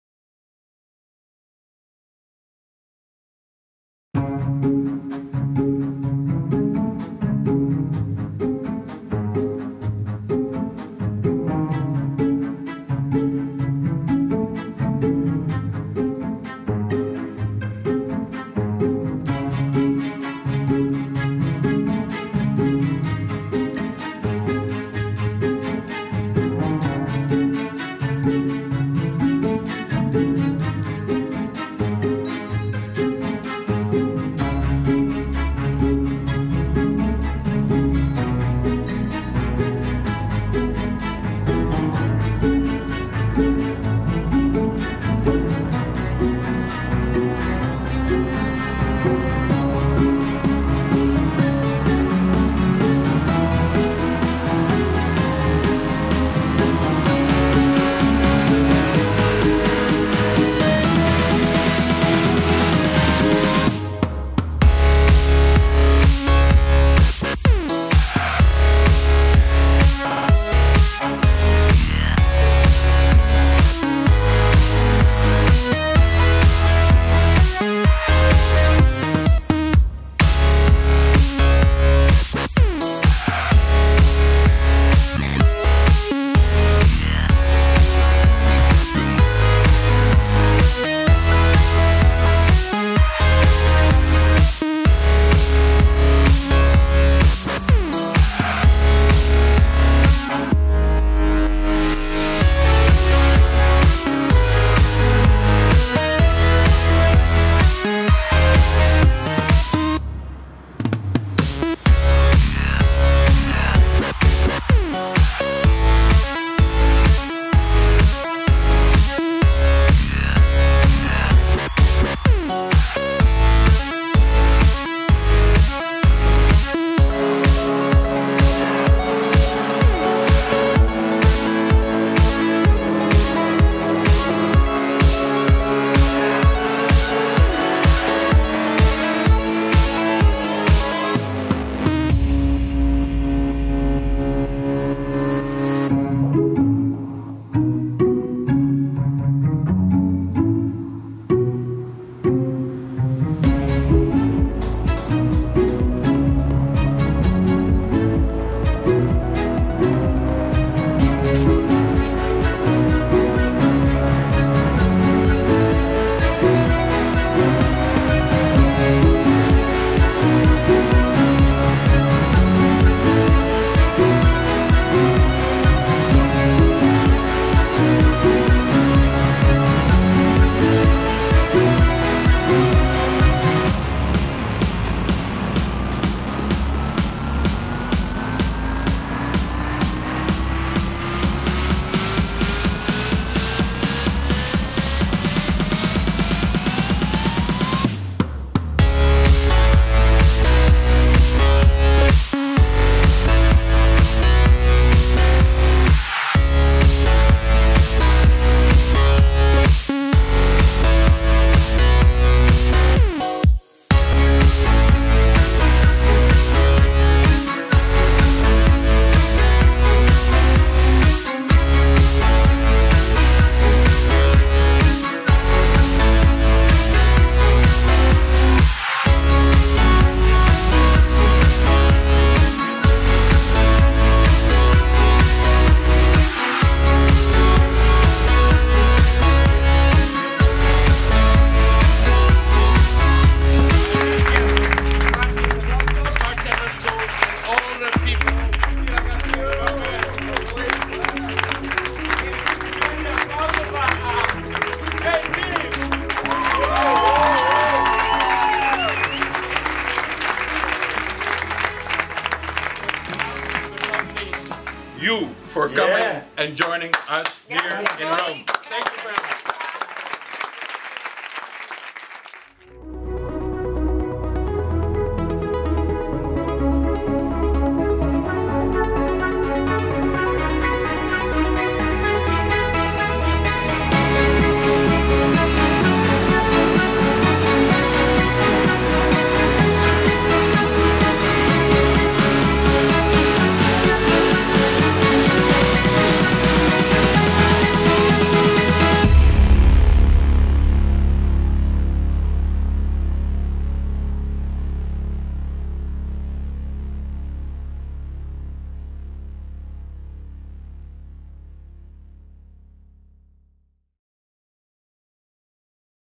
De Kuyper A Team in Party in Bottle Flair Bartending School
Party in Bottle Bartending School di Roma è stata lieta di ospitare uno dei Masterclass più interessanti del 2013, in collaborazione con il Gruppo Pernod Ricard.
Nella sede centrale di Via della Moletta 15, nel cuore della Garbatella, storico quartiere romano, si sono succeduti dietro il banco bar della scuola quattro personaggi del panorama mondiale del Bartending. "The Brain" Matthijs Janssen, Master Distiller DeKuyper, "Mr. C" Chioke Rosalia, Bar Manager internazionale, "The Fixer" Arno Van Eijmeren, mixologist Olandese ed il quattro volte campione del mondo, che non ha bisogno di presentazioni, Nicolas Saint-Jean, della Flair Motion.
Davanti ad oltre settanta bartenders della scuola, i quattro hanno dato vita ad un magico pomeriggio tra amicizia, degustazioni e spettacolo ... in un clima di festa!